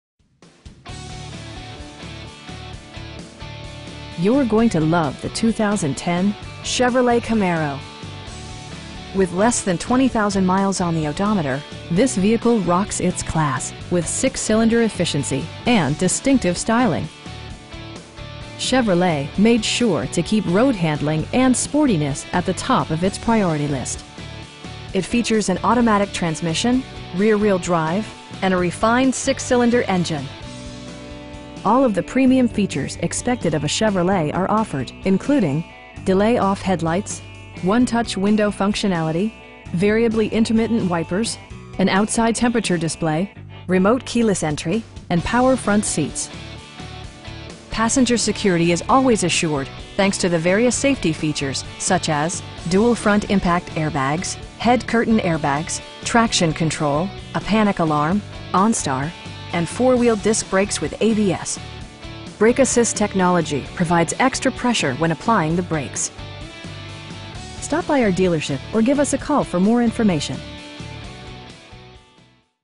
2010 Chevrolet Camaro LT in Oklahoma City, OK 73114
Bob Moore Cadillac Saab of Edmond
13000 N. Broadway Extension

You're going to love the 2010 Chevrolet Camaro. With less than 20,000 miles on the odometer, this vehicle rocks its class with 6-cylinder efficiency and distinctive styling! Chevrolet made sure to keep road-handling and sportiness at the top of it's priority list. It features an automatic transmission, rear-wheel drive, and a refined 6 cylinder engine. All of the premium features expected of a Chevrolet are offered, including: delay-off headlights, 1-touch window functionality, variably intermittent wipers, an outside temperature display, remote keyless entry, cruise control, and power front seats. Passenger security is always assured thanks to various safety features, such as: dual front impact airbags, head curtain airbags, traction control, a panic alarm, onStar, and 4 wheel disc brakes with ABS. Brake assist technology provides extra pressure when applying the brakes. We pride ourselves on providing excellent customer service. Stop by our dealership or give us a call for more information.